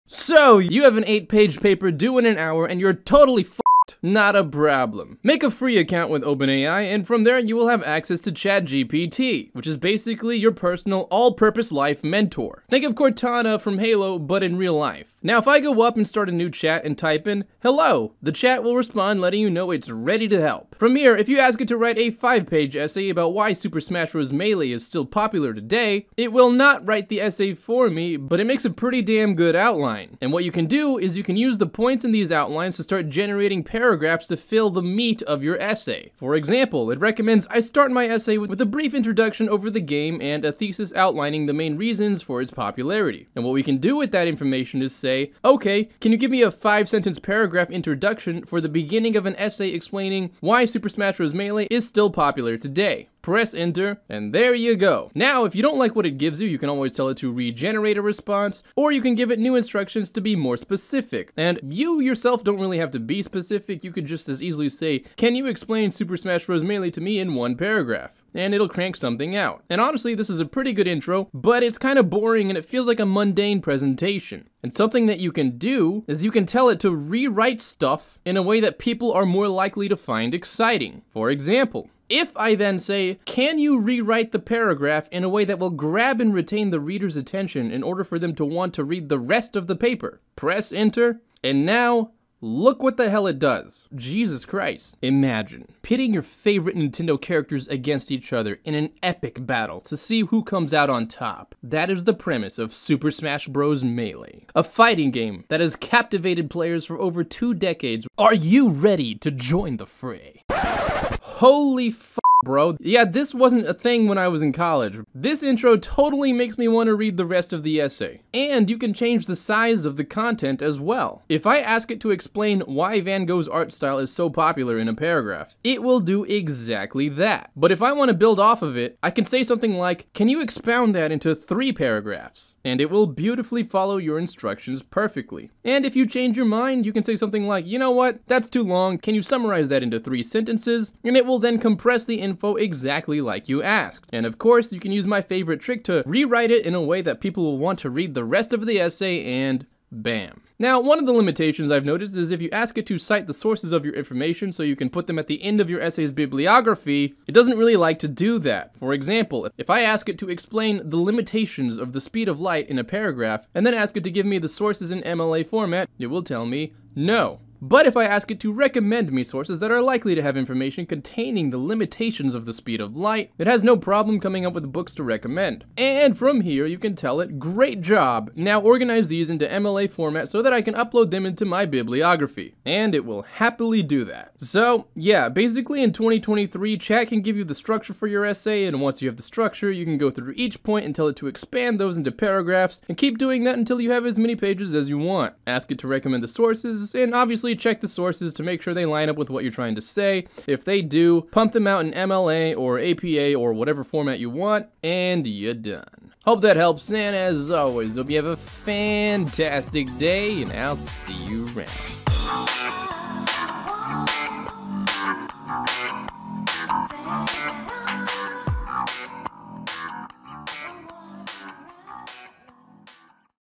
So - Your Essay Is DUE in 1 HOUR?!
Here's the easiest way to generate ANY essay or school paper in 2023 -